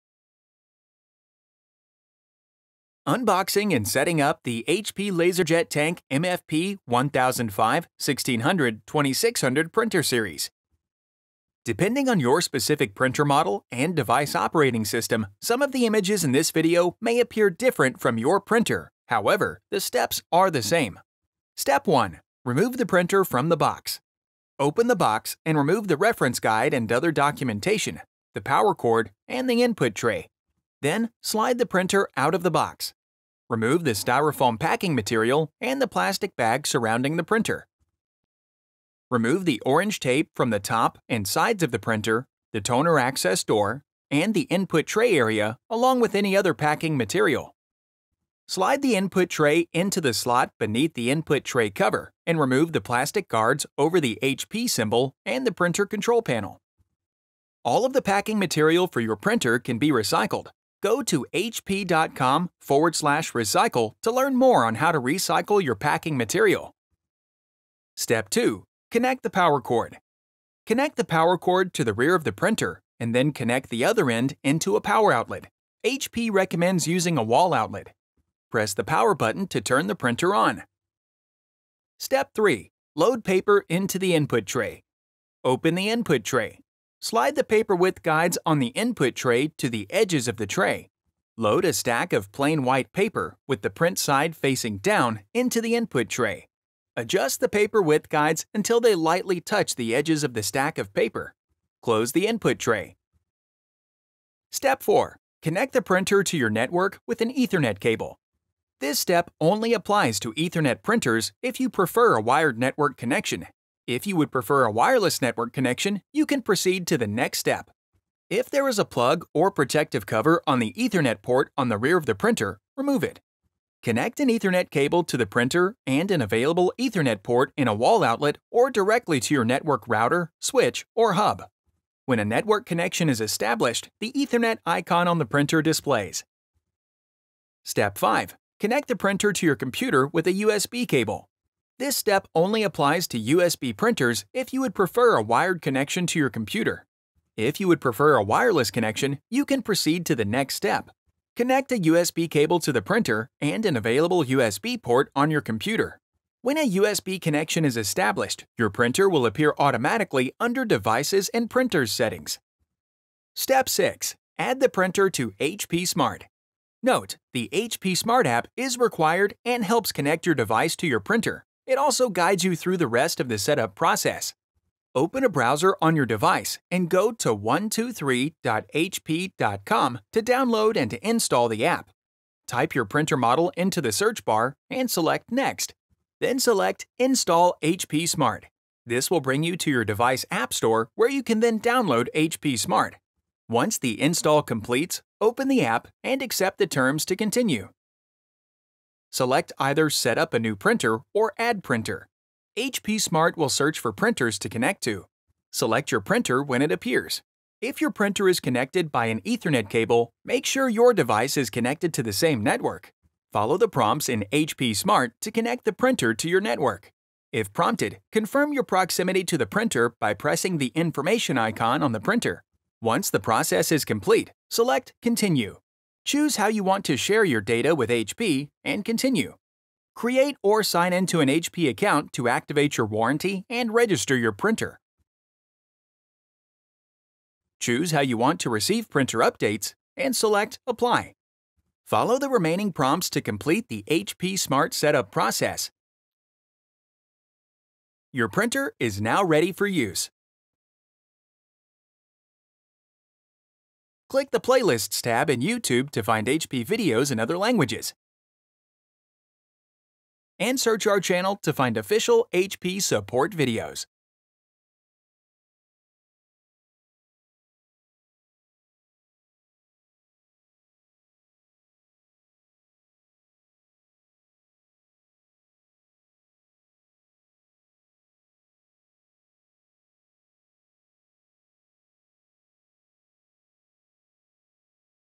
How to Unbox HP LaserJet Tank MFP 1005, 1600, 2600 Printers & Connect to a Wi-Fi or Wired Network
Learn how to unbox your HP LaserJet Tank printer and connect it to a wireless network, a wired network using an Ethernet connection, or to your computer using a USB connection. This video shows how to use the HP Smart app to quickly set up your printer using your cell phone, computer, or tablet and applies to the following printer models:
HP LaserJet Tank MFP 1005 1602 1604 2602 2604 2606 Printers

Chapters:
00:00 Introduction
00:22 Remove the Printer from the Box
01:07 Connect the Power Cord
01:21 Load Paper into the Input Tray
01:44 Connect Printer to Network with Ethernet Cable
02:17 Connect Printer with USB Cable
02:42 Connect the Printer to Wi-Fi with HP Smart app

How to Unbox HP LJ Tank MFP1005,1600,2600 Printers & Connect to Wi-Fi or Wired Network | HP Support